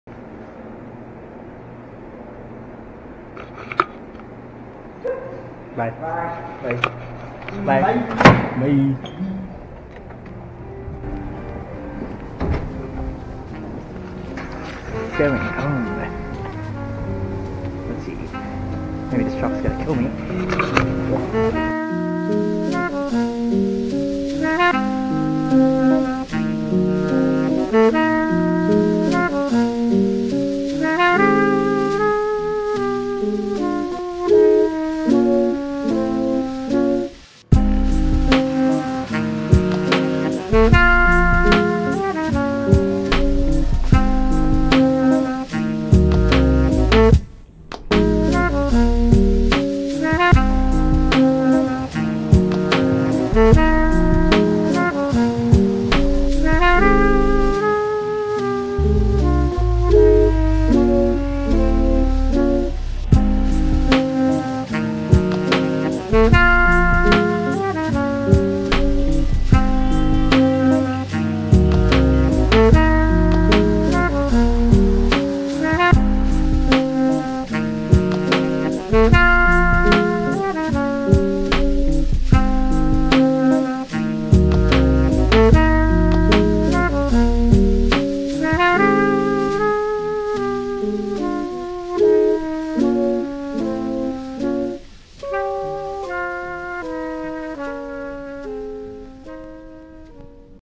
After Work Christmas Feels
Just a small Christmasy vibe video I made. Shops at Don Mills has an interesting Christmas decoration setup for the holidays.